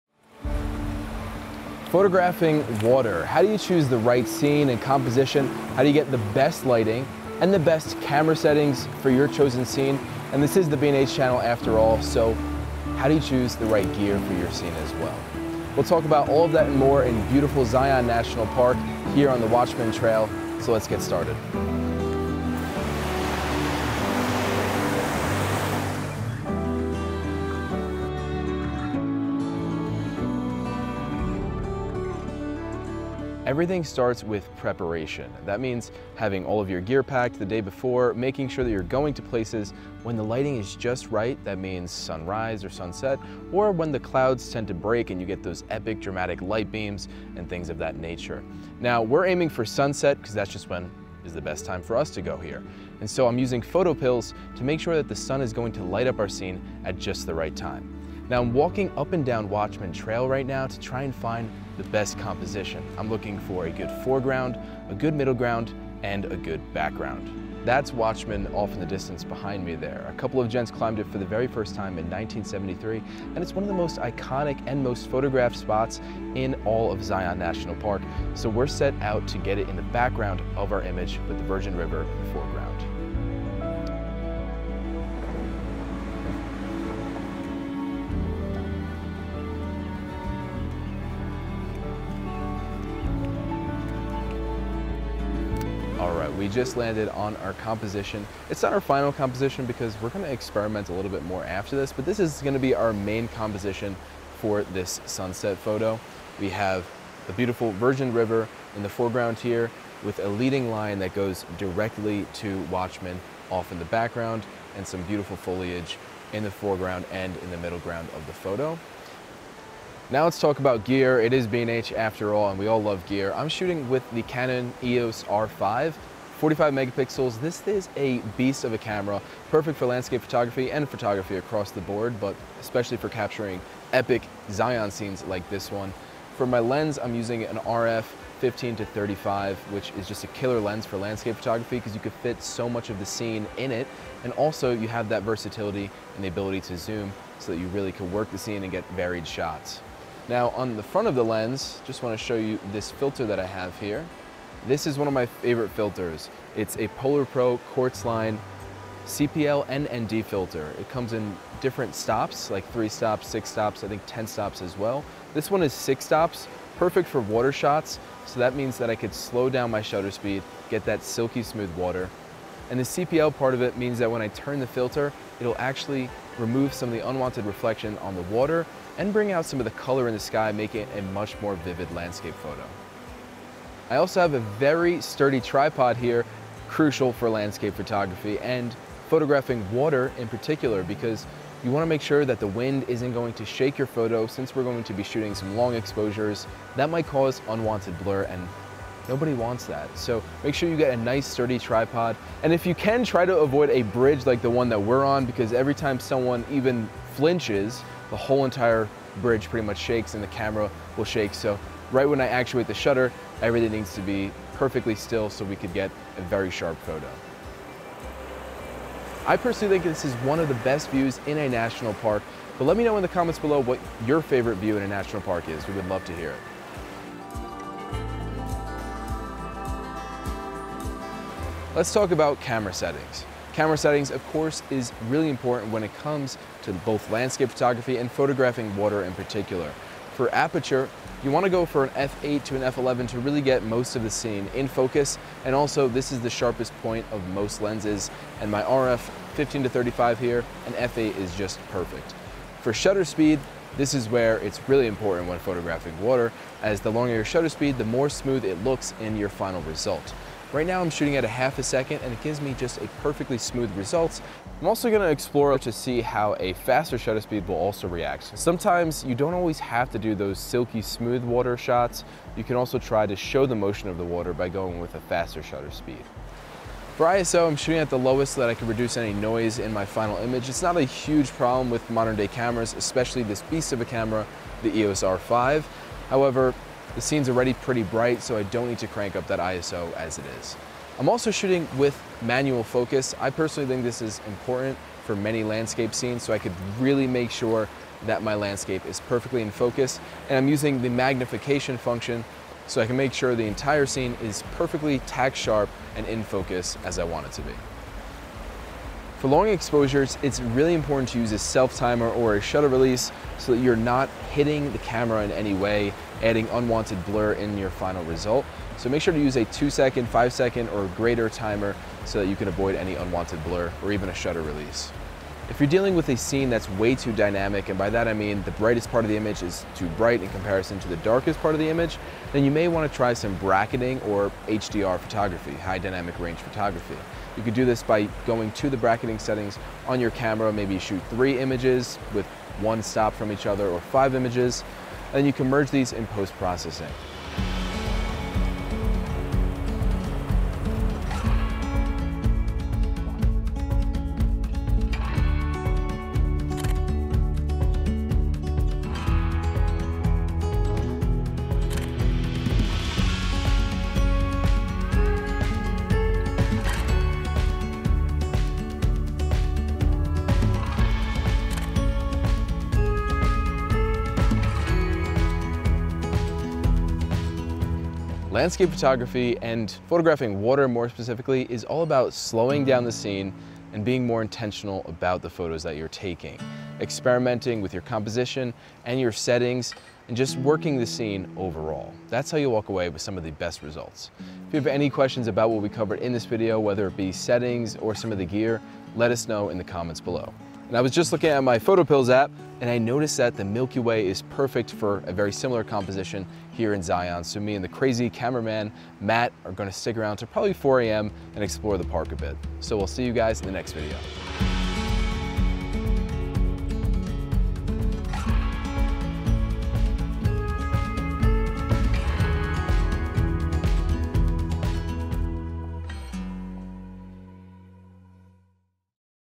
Photographing Water in Landscapes | Outdoors with B&H
Matt is on location in Zion National Park to teach you how to photograph water in landscapes. You’ll learn how to choose your composition, recommended gear and settings to choose when photographing moving water.

Gear used
- Canon EOS R5 Mirrorless Camera
- Canon RF 15-35mm f/2.8 L IS USM Lens
- PolarPro 82mm ND64 QuartzLine Solid Neutral Density 1.8 and Circular Polarizer Filter (6-Stop)
- Oben CT-3535 Folding Carbon Fiber Travel Tripod with BE-208T Ball Head
- Atomos Ninja V 5" 4K HDMI Recording Monitor

0:00 In this video
0:32 Finding Your Composition
1:59 Gear
3:51 Settings
5:56 Matt's Images
6:18 Final Thoughts